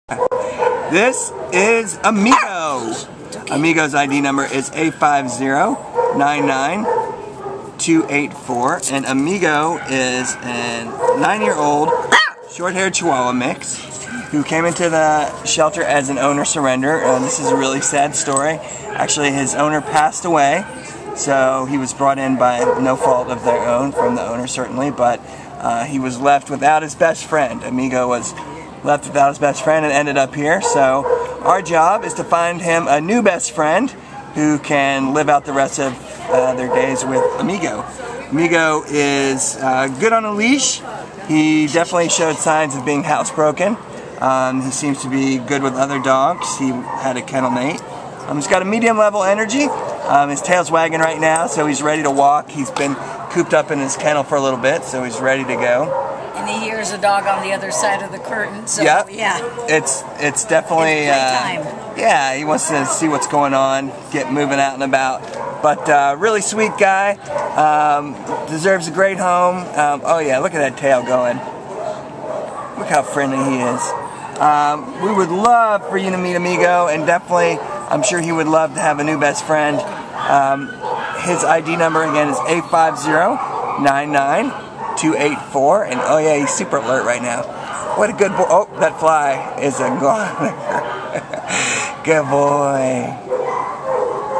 A5099284 Amigo | Chihuahua
A5099284 Amigo is a precious and spunky 9-year-old tricolor (black, tan, caramel) neutered male Chihuahua who was surrendered at the Baldwin Park Animal Care Center on August 11th because his owner died. Weighing 8 lbs, Amigo is good on leash, shows signs of being housebroken, has a nice medium energy, and gets along great with his same-size kennel mates. In the yard, he pranced around on leash and wanted to make friends with all the other littles! He’d walk up easily, wag his tail, and gently introduce himself—it was a joy to watch! He also loves being with people; he’s a gregarious guy! We think this sparkly-eyed and loving little man will make a great indoor pet and companion for just about anyone (as long as small children are careful of his tiny size)